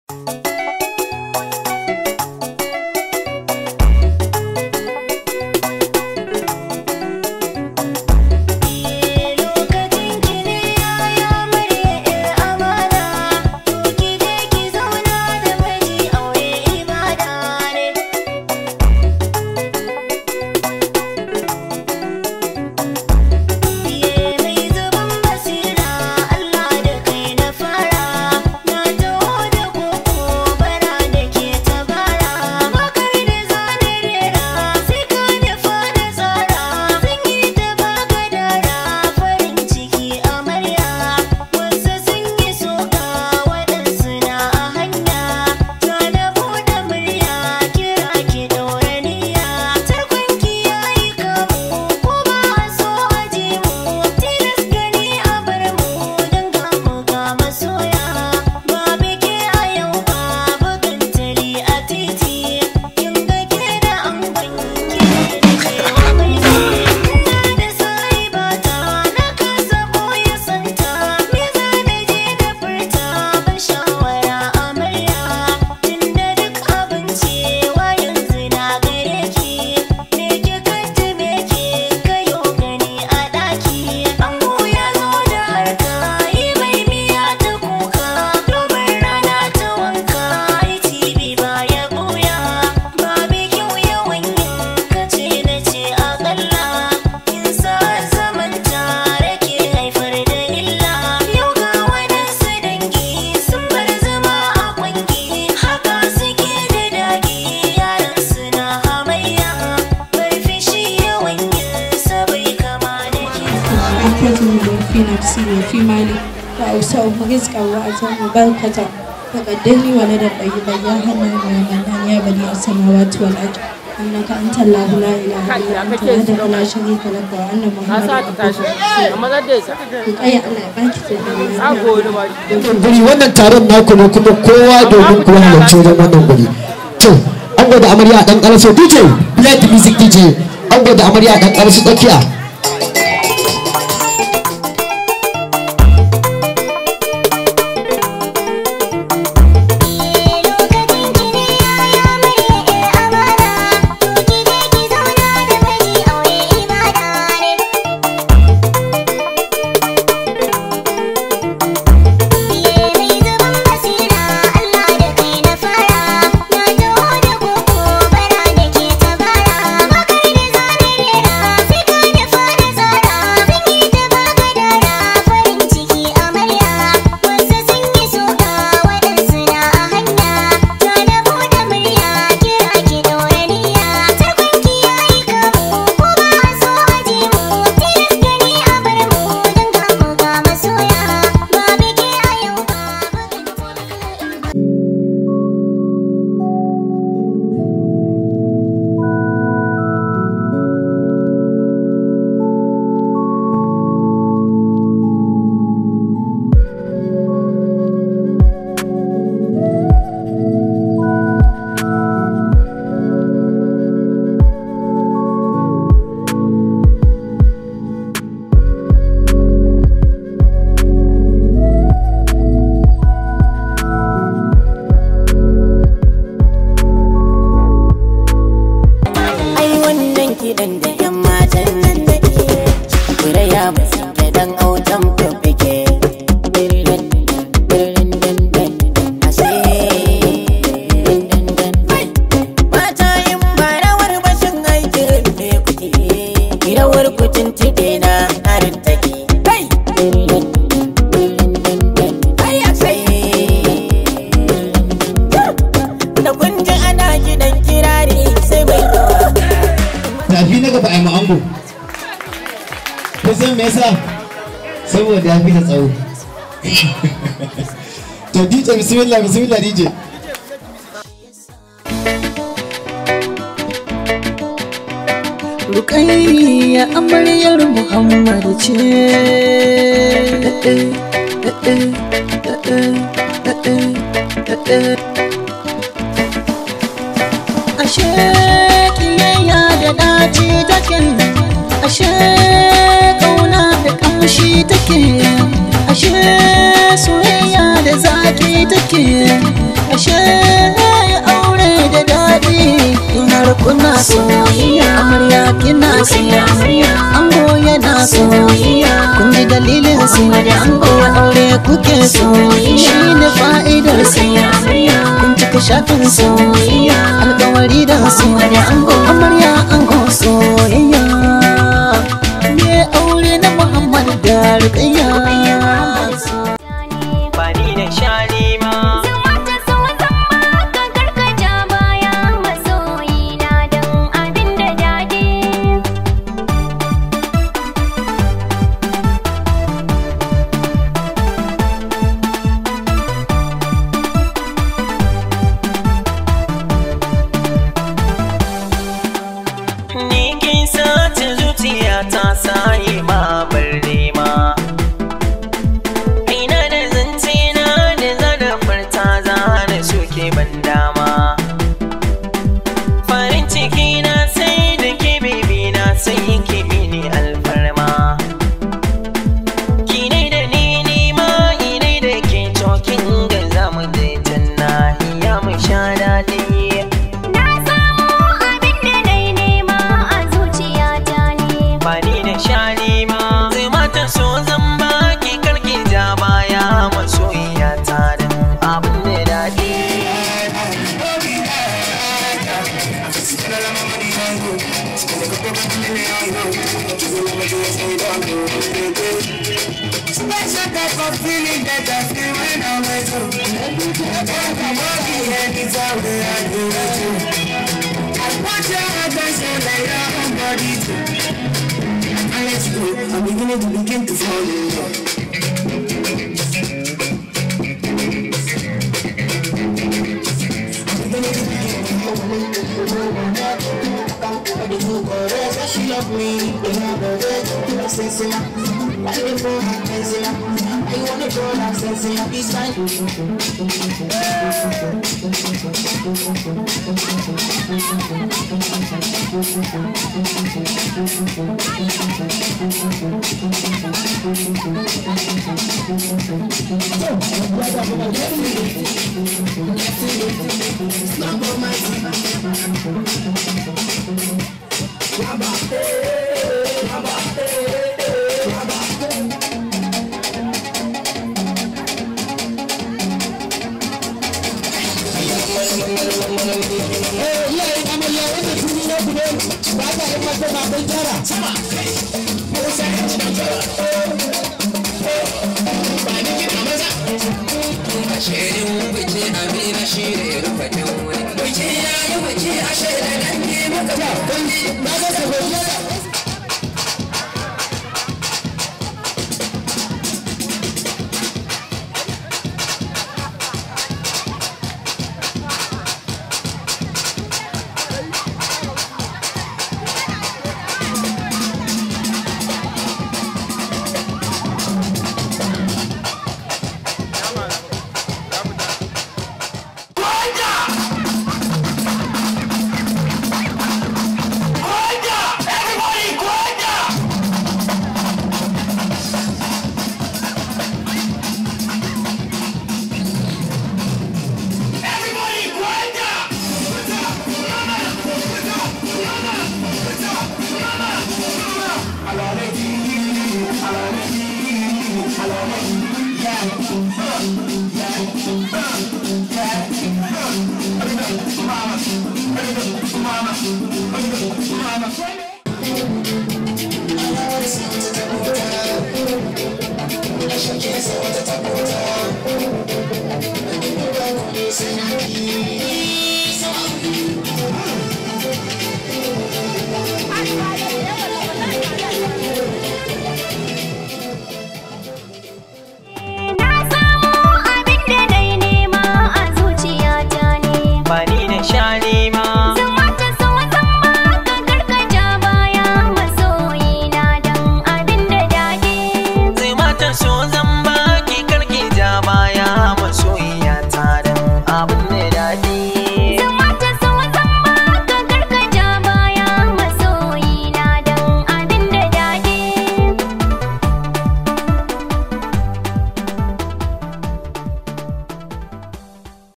Rukayyah Weds Muhammad 2021
Auren Rukayyatu da Muhammad (2021) ¬ Hussaini Danko ya yi performance a wurin Taron.